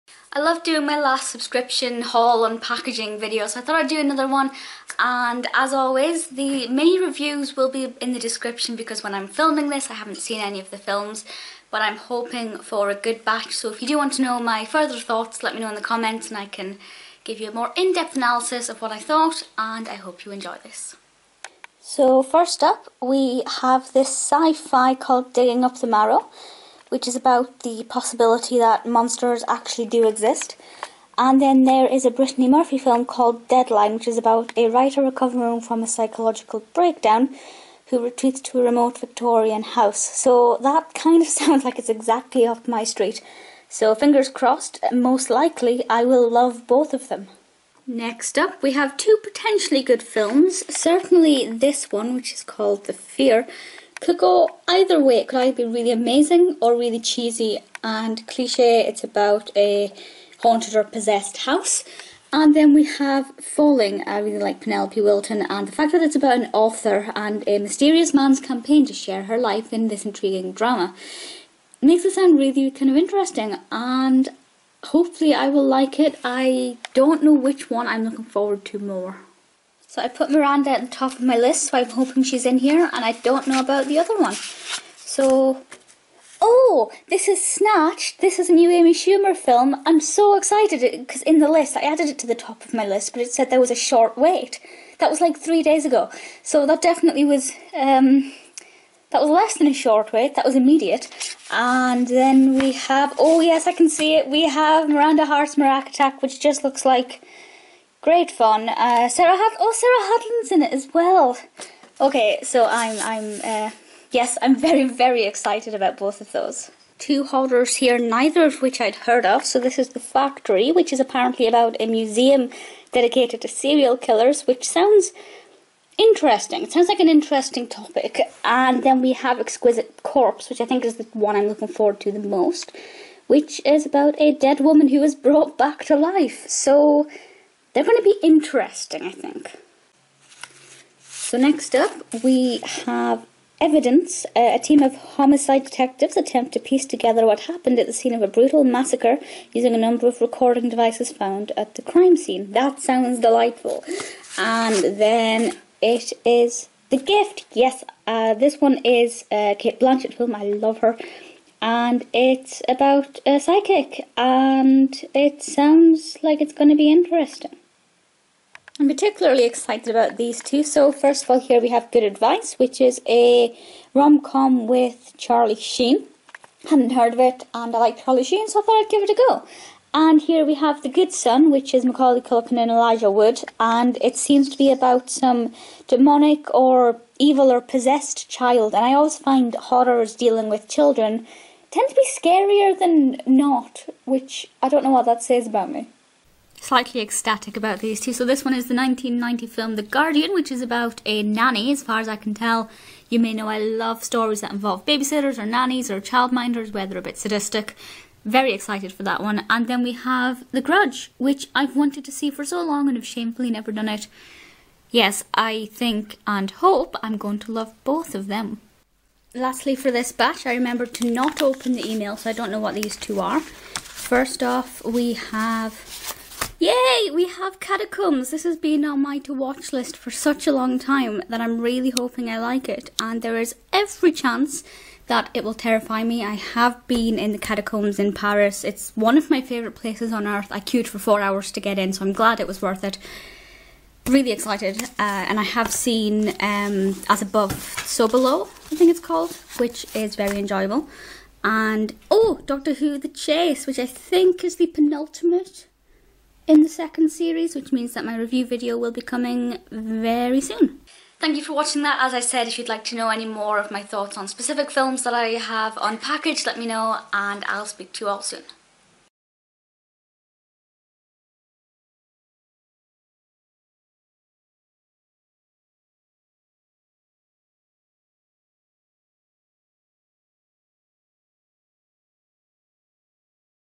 UK Film Subscription Haul! Three of the Best Films EVER?!
MY BOOKS Film Subscription Haul! Three of the Best Films I've Ever Seen?! [OPEN FOR LINKS AND INFO!] unboxing movie subscription Welcome to my latest movie subscription haul, which features three of the best films I've ever seen! Scroll down for the list of movies mentioned. These DVDs are from subscription service Cinema Paradiso

Amy McLean

Digging Up the Marrow 2014
Deadline 2009
The Fear/Perfect House 2013
Falling 2005
Snatched 2017
The Factory/Death Factory 2014
Exquisite Corpse 2010
Evidence 2013
The Gift 2000
The Good Son 1993
Good Advice 2001
The Guardian 1990
The Grudge 2004
Catacombs 2007
Miranda Hart Maracattack